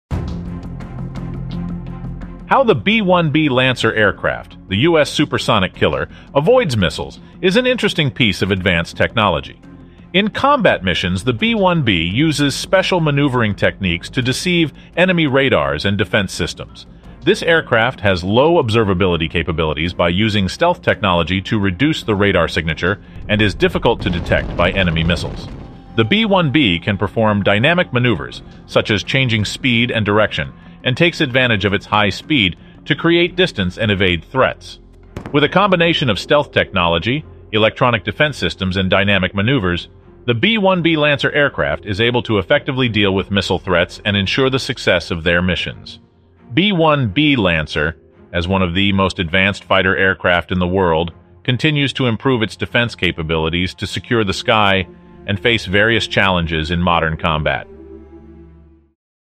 How the B-1B Lancer Avoids Missiles
This aircraft has low observability capabilities by using stealth technology to reduce the radar signature and is difficult to detect by enemy missiles. The B-1B can perform dynamic maneuvers such as changing speed and direction, and takes advantage of its high speed to create distance and evade threats.